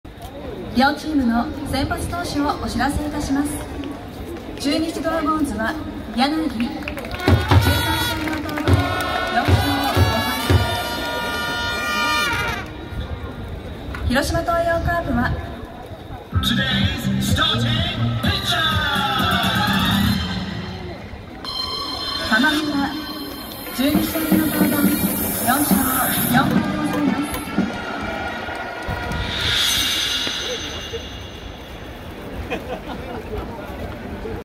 20240908 玉ちゃん柳！今日本日･先発投手の発表【広島東洋カープvs中日ドラゴンズ】試合前@MAZDA Zoom-Zoom ｽﾀｼﾞｱﾑ広島･ﾋﾞｼﾞﾀｰﾊﾟﾌｫｰﾏﾝｽ
【広島東洋ｶｰﾌﾟ】玉村昇悟
【中日ﾄﾞﾗｺﾞﾝｽﾞ】柳裕也